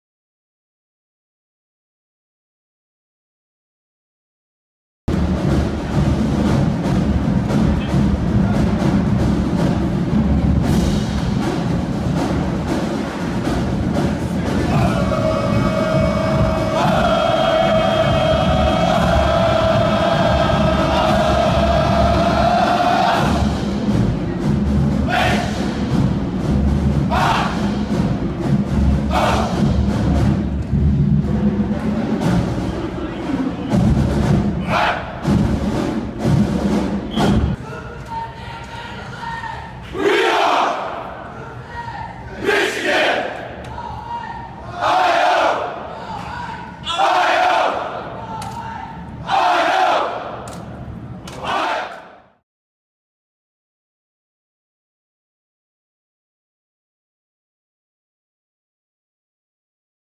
Ohio State Football vs Michigan 2016
Video and still photo footage that I shot on my iPhone 7 Plus before, during and after "The Game".